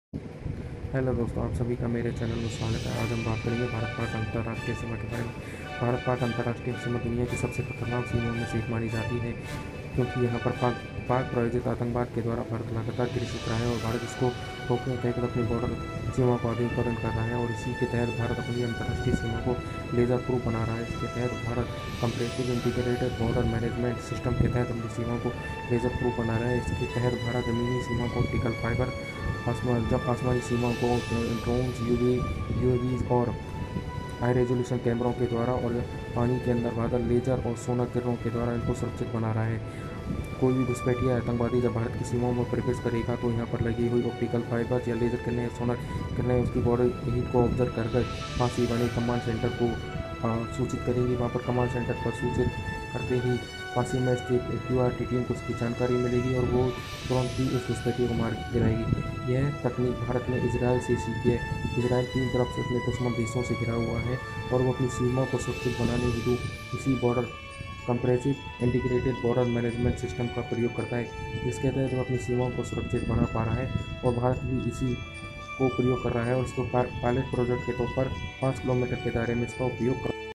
भारत ने सीमा !! पर खड़ी की !! अदृश्य दीवार !! Smart border fancing
भारत ने भारत पाक सीमा को अभेद बनाने हेतु !! सीमा पर लेजर दीवार खड़ी की है !! जिसका मकसद आतंकबाद को रोकना ! सीमाओं को सुरक्षित बनाना है !